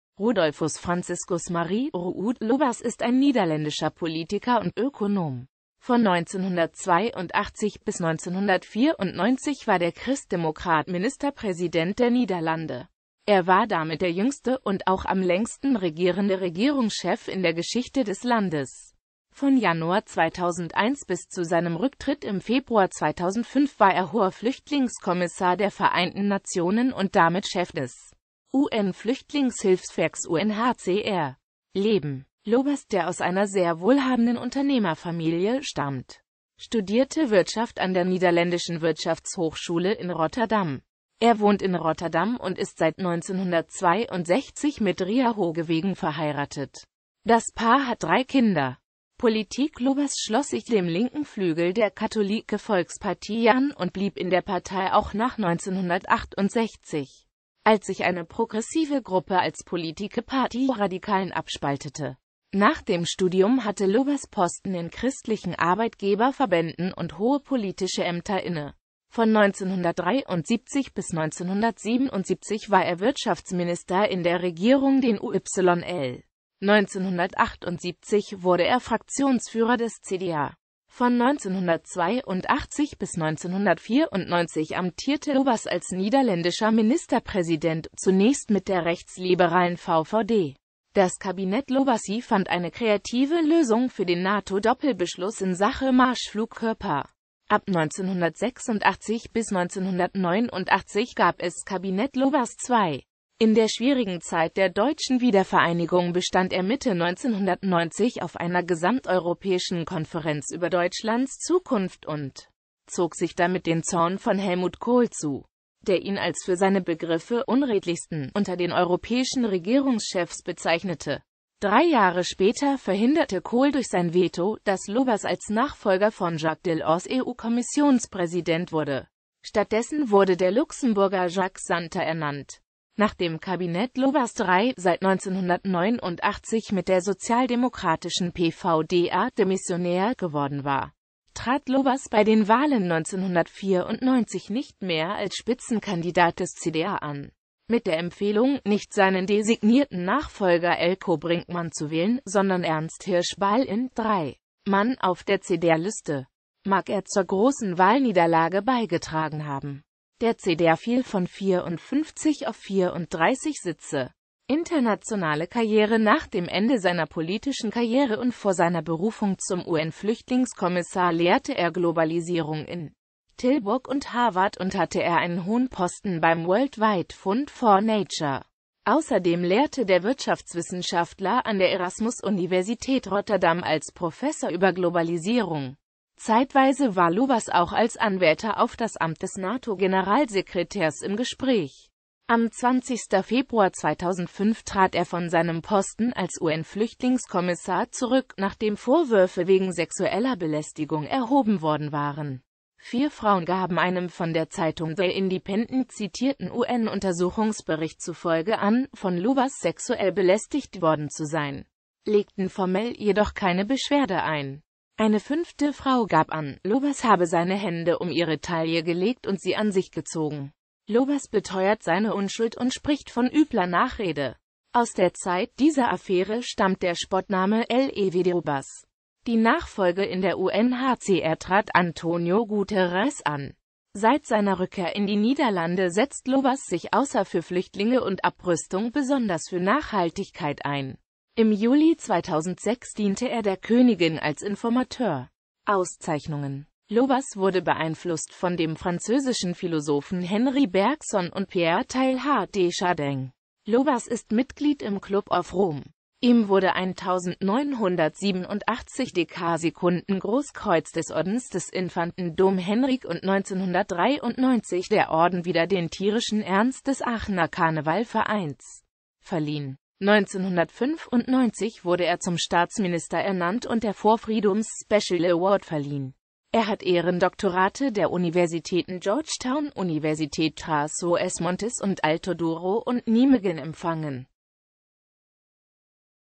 Ruud Lubbers
Vielen Dank für Ihre Unterstützung:

Rudolphus Franciscus Marie „Ruud“ Lubbers (* 7.Mai 1939 in Rotterdam) ist ein niederländischer Politiker und Ökonom.Von 1982 bis 1994 war der Christdemokrat Ministerpräsident der Niederlande.Er war damit der jüngste und auch am längsten regierende Regierungschef in der Geschichte des Landes.

Bild-Copyright-Informationen
Urheber Info: Rijksoverheid.nl
Lizenz: Creative Commons Zero, Public Domain Dedication (CC0)